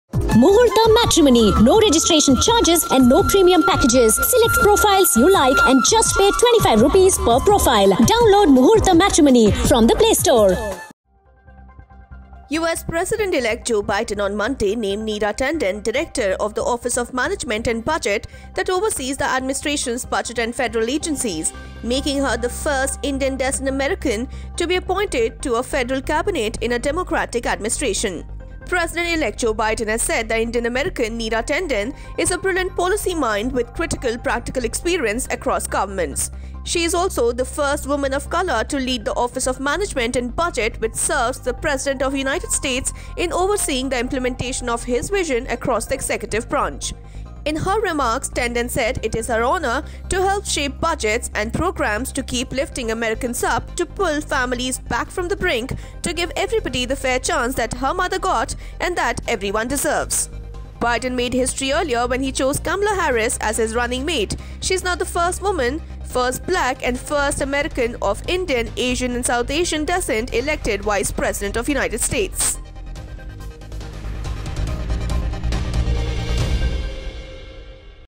Neera Tanden is the first Indian-Americana and a woman of colour to be picked|Oneindia News
US President-elect Joe Biden on Monday named Neera Tanden director of the Office of Management and Budget (OMB) that oversees the administration’s budget and federal agencies, making her the first Indian-descent American to be appointed to a federal cabinet in a Democratic administration. President-elect Joe Biden has said that Indian-American Neera Tanden is a brilliant policy mind with critical practical experience across governments. She is also the first woman of coulour to lead the Office of Management and Budget. Biden made history earlier when he chose Kamala Harris his running mate. She is now the first woman, first black and first American of Indian, Asian and South Asian descent elected vice-president.